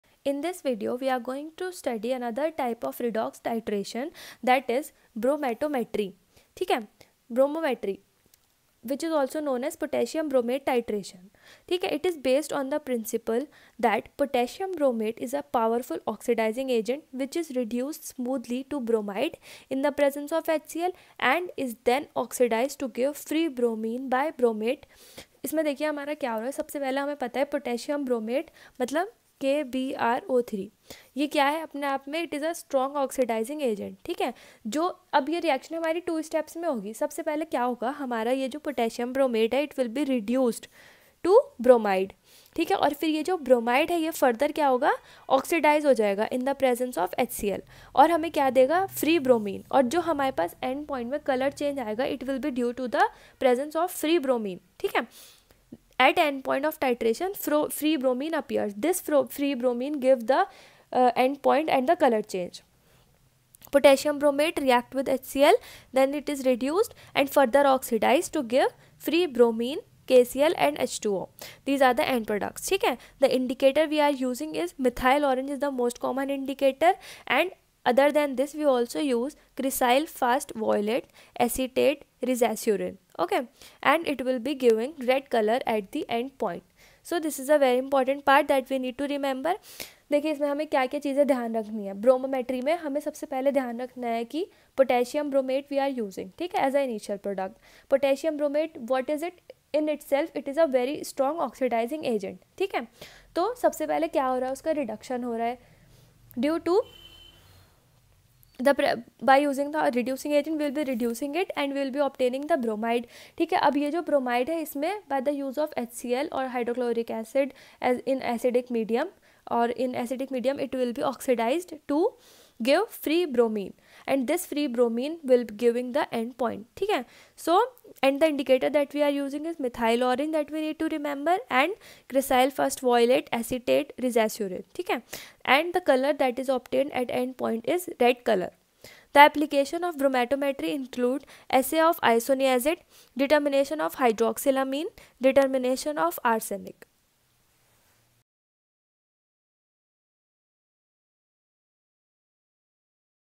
Bromometry | Principle | Applications | Easy handwritten notes and explanation for exams
This video comprise of detailed explanation of iodometry.